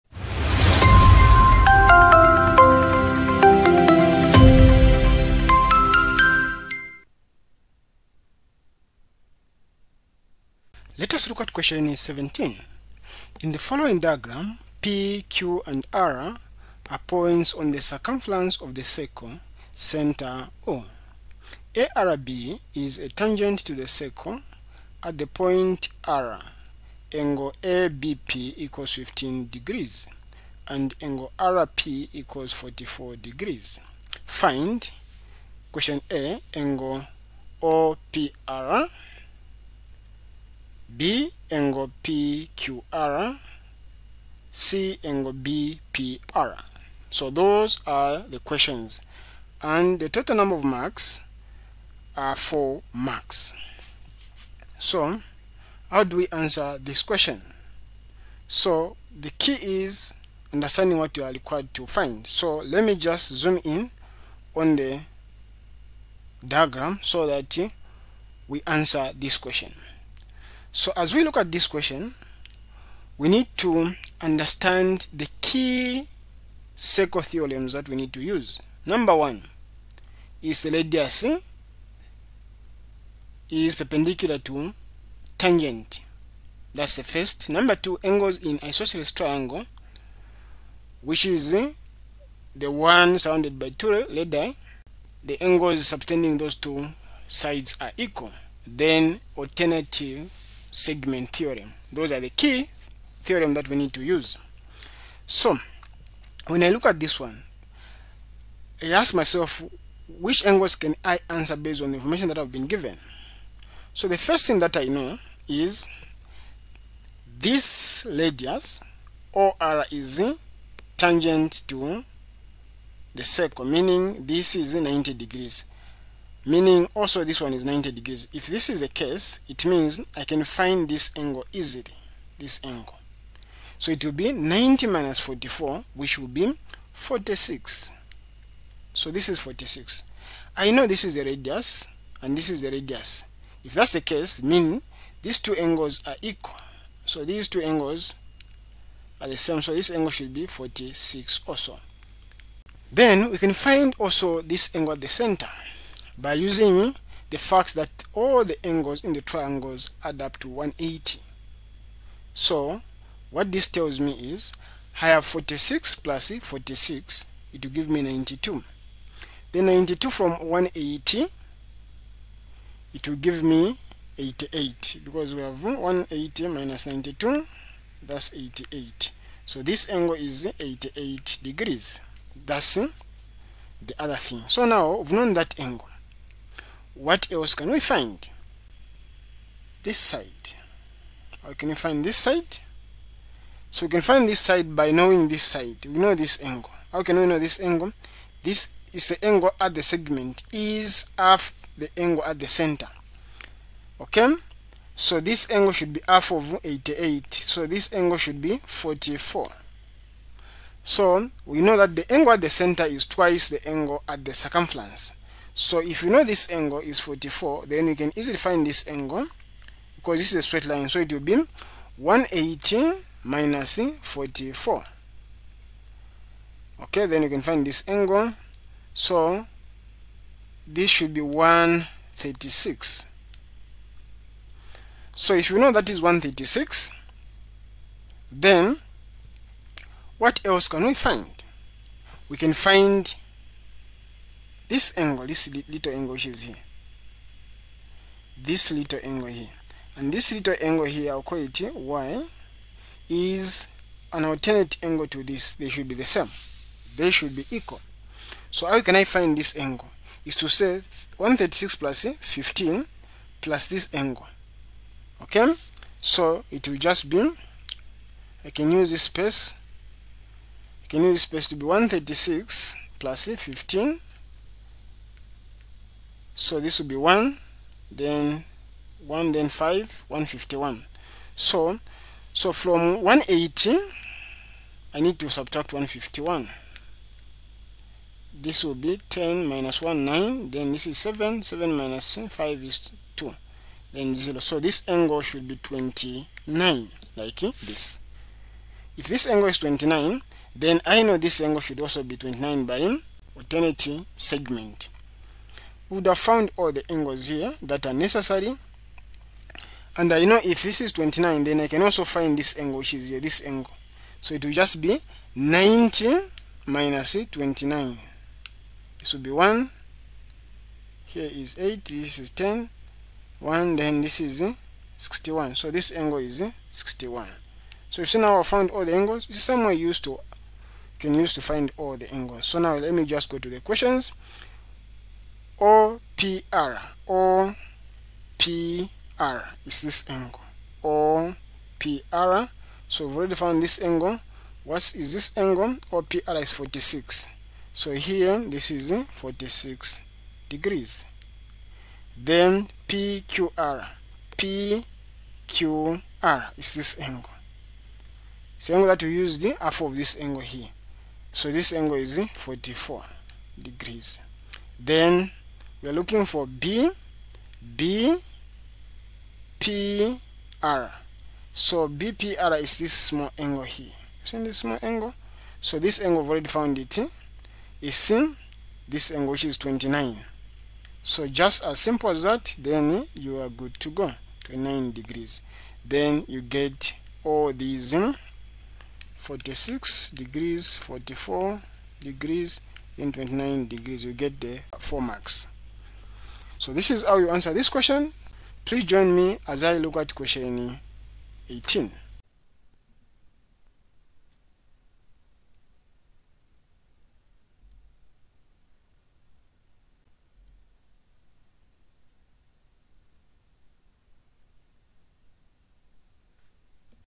2024 Internal Mathematics Paper 1 Question 17
Are you struggling to understand the ECZ Mathematics Paper 2? Look no further! In this video, we'll break down each question step-by-step, providing clear explanations and helpful tips.

Join our WhatsApp group for instant updates, get an the Study Companion, discussions, and support.